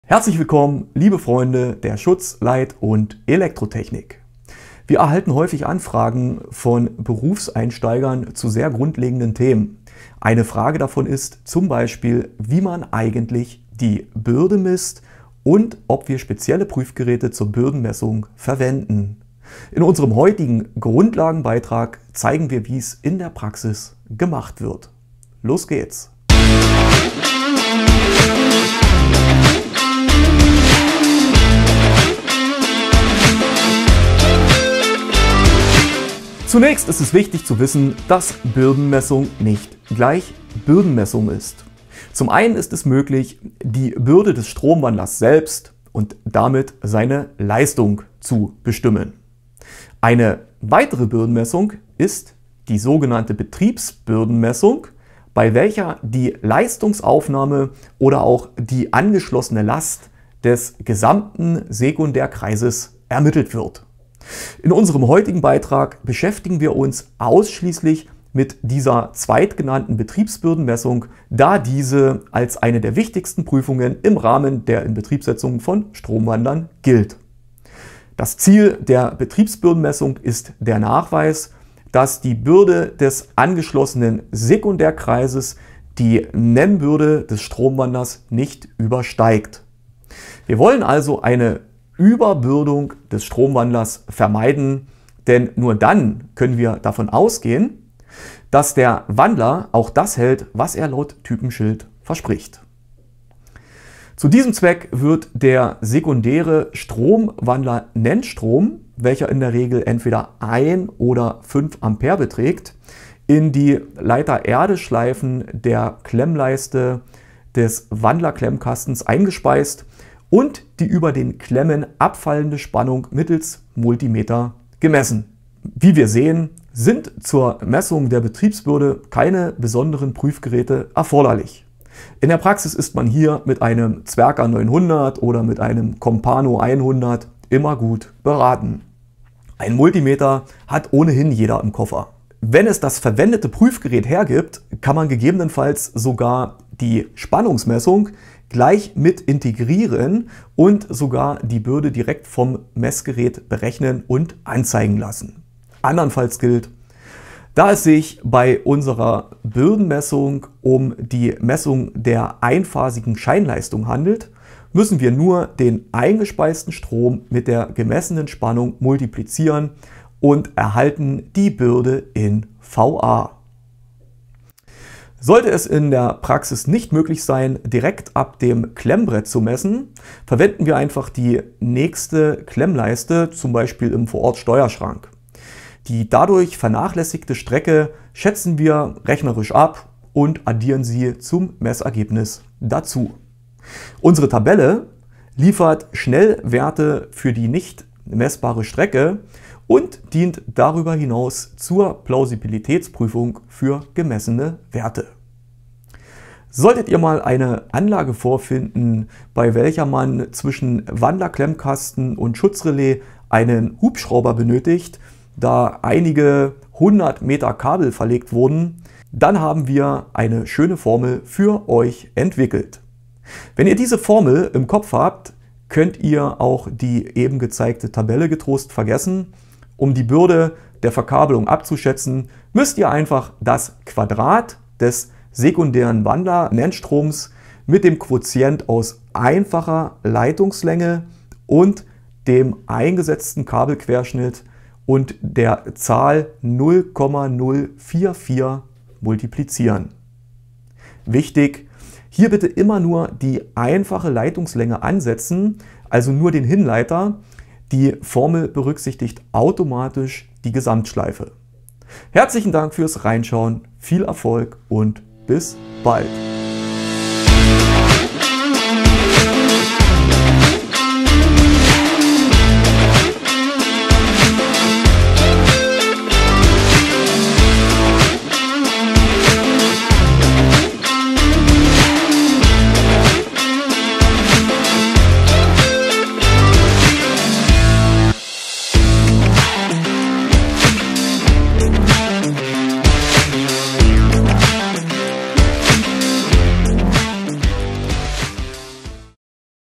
Wie misst man eigentlich Bürde?
HERZlich Willkommen, liebe Freunde der Schutz-, Leit- und Elektrotechnik. Wir erhalten häufig Anfragen von Berufseinsteigern, zu sehr grundlegenden Themen. Eine Frage davon ist zum Beispiel, wie man eigentlich die Bürde misst und ob wir spezielle Prüfgeräte zur Bürdenmessung verwenden. In unserem heutigen Grundlagenbeitrag zeigen wir wie’s in der Praxis gemacht wird.

Los geht’s!